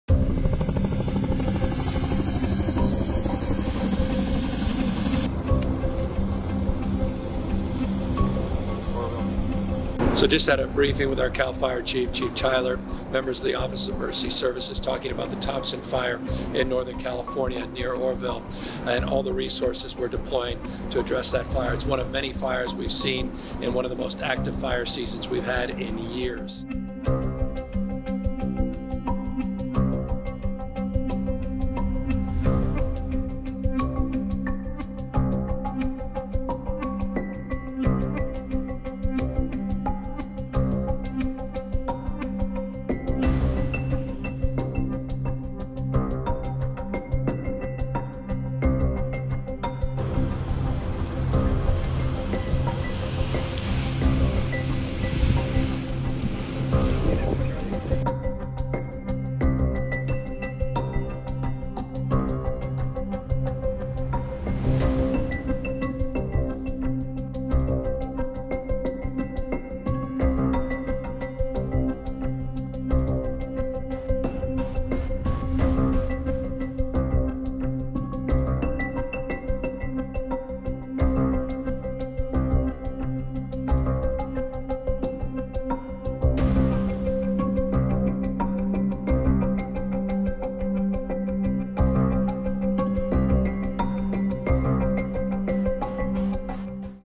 Governor proclaims State of Emergency in response to the Thompson Fire in Northern California
California Gov. Gavin Newsom proclaimed a state of emergency Wednesday morning in response to the Thompson Fire burning in Northern California.

On Tuesday evening, July 2, the City of Oroville announced a local emergency amid the fire burning in Butte County, prompting thousands to evacuate. The vegetation fire ignited near Cherokee Road and Thompson Flat Cemetery Road at 10:51 a.m. on Tuesday, according to Cal Fire's Butte Unit.

As of 11:45 a.m. Wednesday, Cal Fire says the Thompson Fire scorched 3,568 acres with no containment.